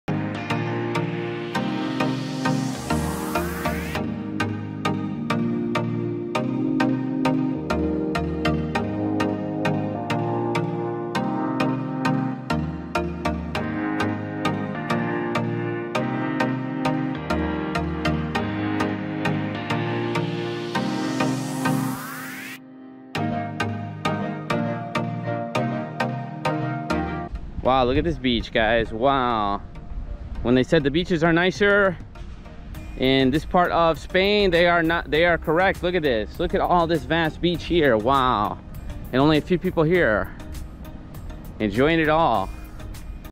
Awesome Beach in Barbate, Spain!
Caños de Meca is an amazing beach in the South West part of Spain. Close to Morocco it is on the Easter part of the Atlantic Ocean.